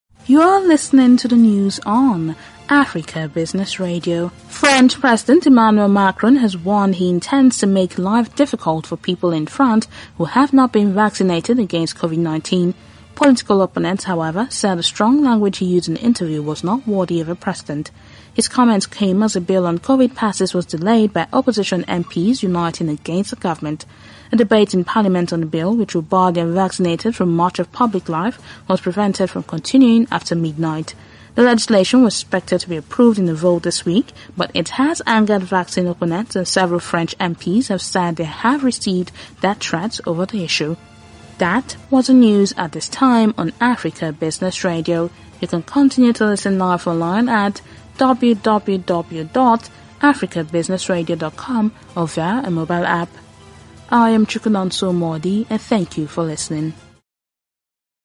President Macron To Pressure Unvaccinated Citizens
French President Emmanuel Macron has warned he intends to make life difficult for people in France who have not been vaccinated against Covid-19.
Political opponents however said the strong language he used in the interview was not worthy of a president.
His comments came as a bill on Covid passes was delayed by opposition MPs uniting against the government.
A debate in parliament on the bill, which would bar the unvaccinated from much of public life, was prevented from continuing after midnight.
The legislation was expected to be approved in a vote this week, but it has angered vaccine opponents and several French MPs have said they have received death threats over the issue.